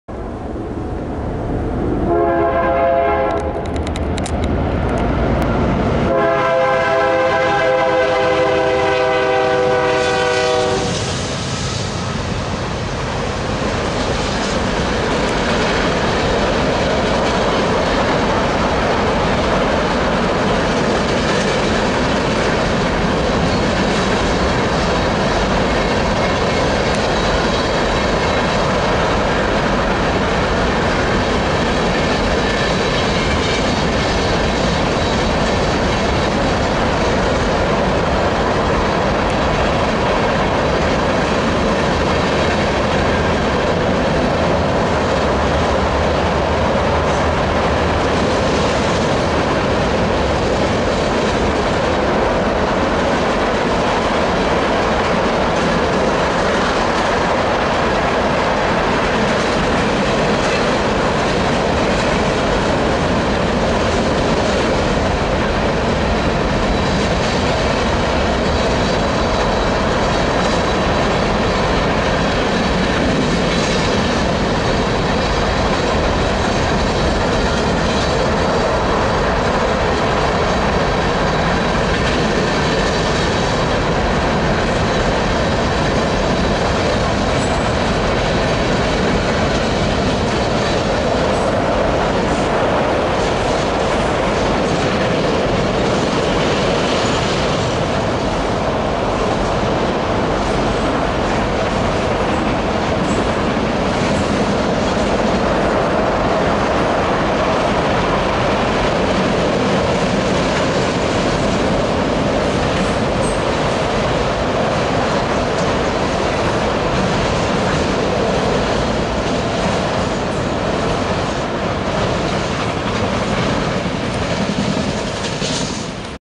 HIGH SPEED BNSF H2 Dash 9 #5224 K3LA & H3 ES44C4 3266 - NS Combined Freight Autorack 309
Here is video I recorded in Unionville Ohio in mid July 2022 of high speed 60MPH NS Norfolk Southern westbound combined freight / autorack train 309 with 136 freight cars + 29 auto carriers = 165 cars / 672 axles, lead by BNSF H2 Burlington Northern Santa Fe C44-9W / Dash 9 #5224 with a Nathan 3 chime K3LA train horn & H3 Tier 4 Credit ES44C4 #3266 trailing 2nd.